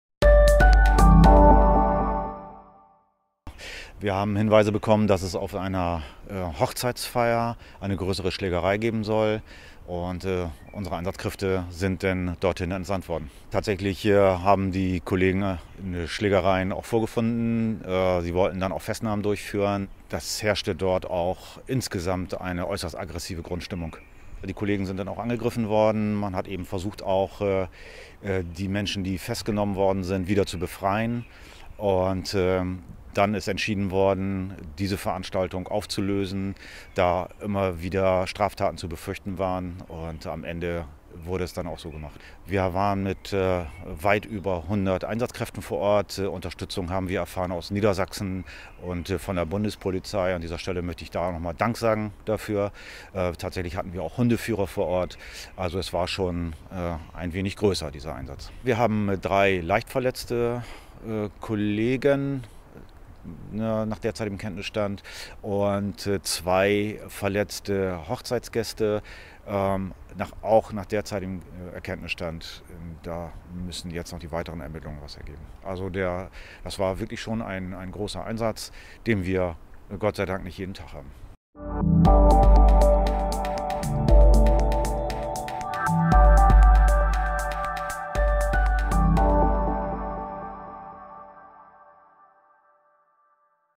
Streit bei Hochzeitsfeier in Bremen eskaliert - Acht Personen festgenommen
In Bremen-Gröpelingen ist ein Streit bei einer Hochzeitsfeier eskaliert und hat zu acht Festnahmen geführt. Laut Polizei vom Sonntagmorgen waren am Vorabend etwa 400 Gäste bei den Feierlichkeiten. Eine Auseinandersetzung uferte im Laufe des Abends so aus, dass die Polizei alarmiert werden musste.

Video: Kai Moorschlatt
Sprecher: Horst Göbel, Pressesprecher Polizei Bremen